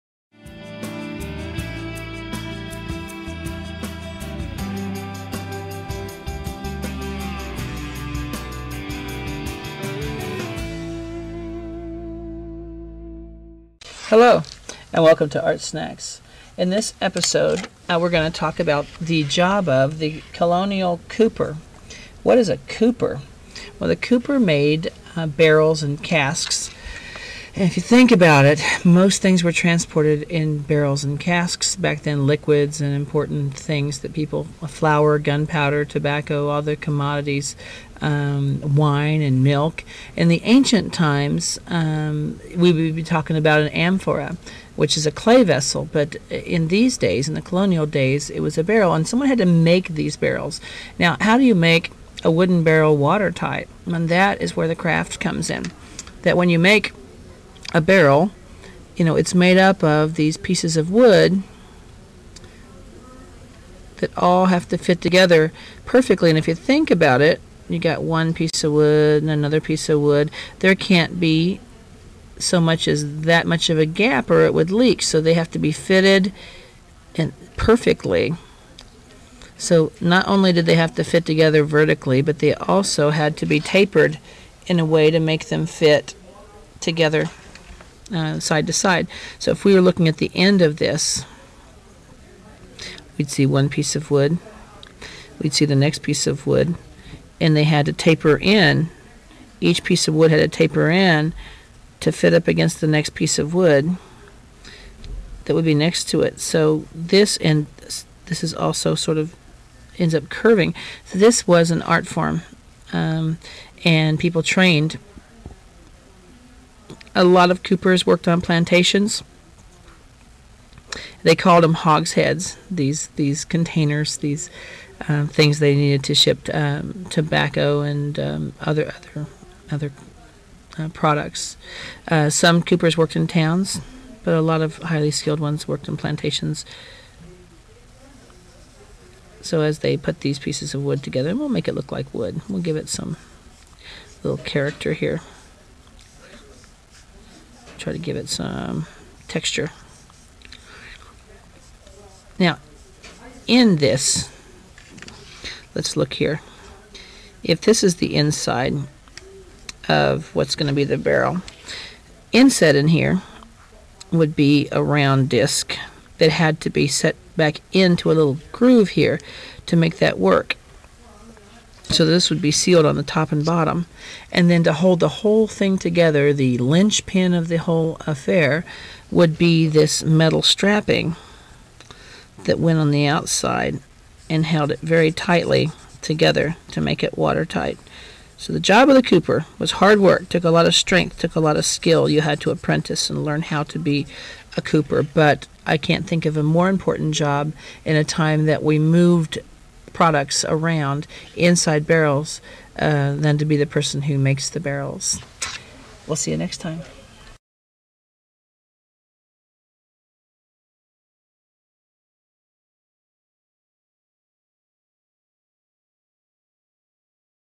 Art Snacks - Colonial Cooper
Kevin teaches about colonial coopers.